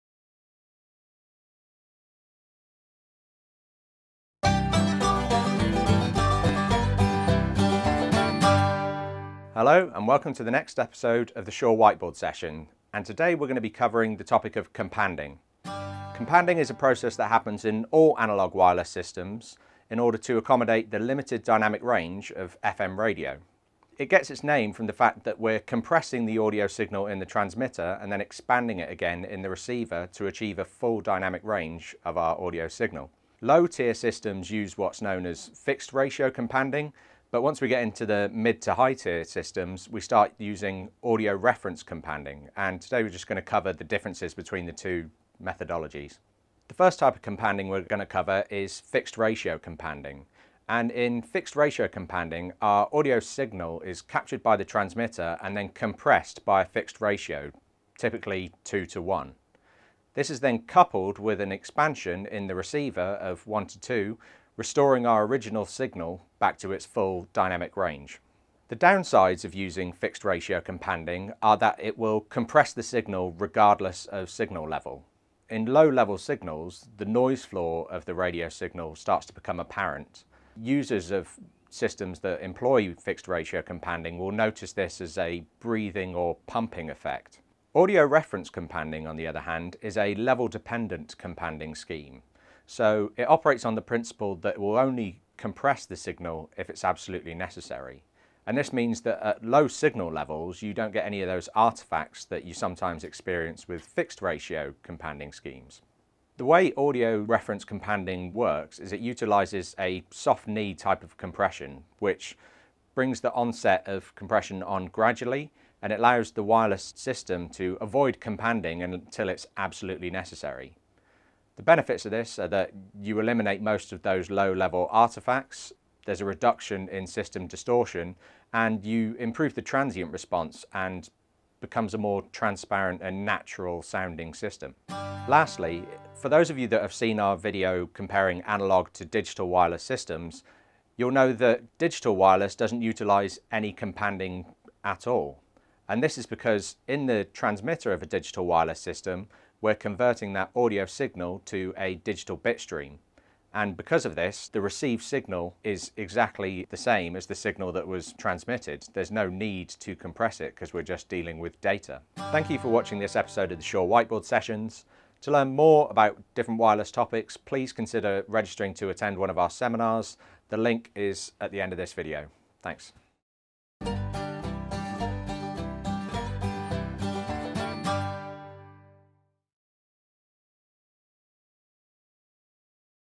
Shure Whiteboard – Wireless System Companding Explained
Senior Product Management Specialist, Stuart Stephens explains wireless companding and how it works.